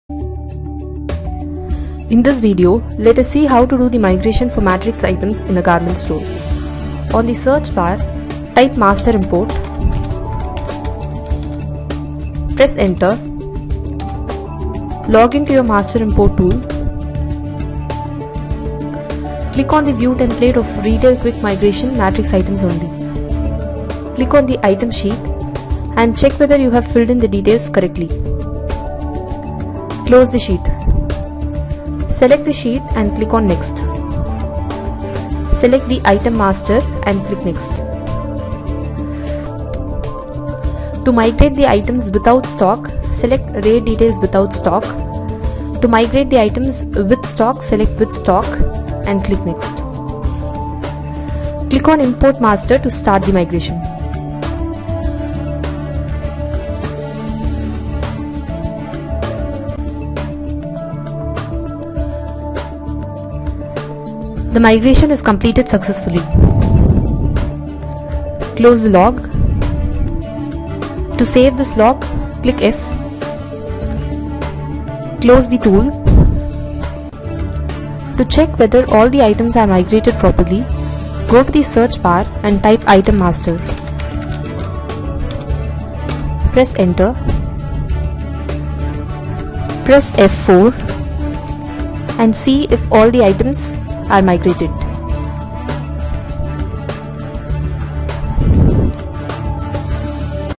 Migration of Data- GoFrugal RPOS7 Garments Product Delivery
Watch this video to know how to do the migration of data in your GoFrugal RPOS7 software.

For further queries, please come over to chat with us @ or raise a ticket via myGoFrugal app, watch how to raise a ticket here